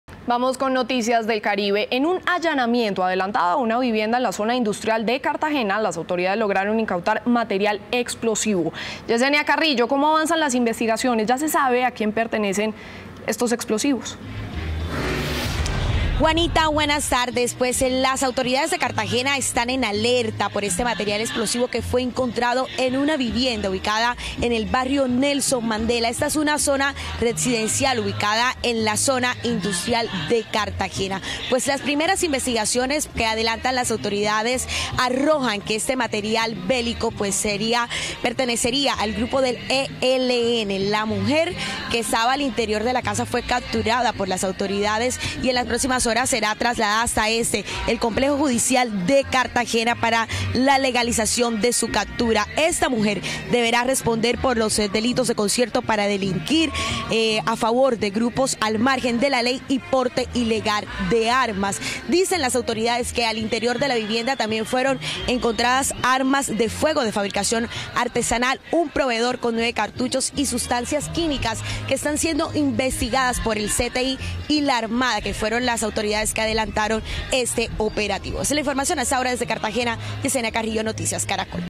Incautan material explosivo que sería del ELN en una vivienda de Cartagena | Noticias Caracol
Uniformados también hallaron armas de fuego de fabricación artesanal y sustancias químicas. Una mujer fue capturada durante la operación.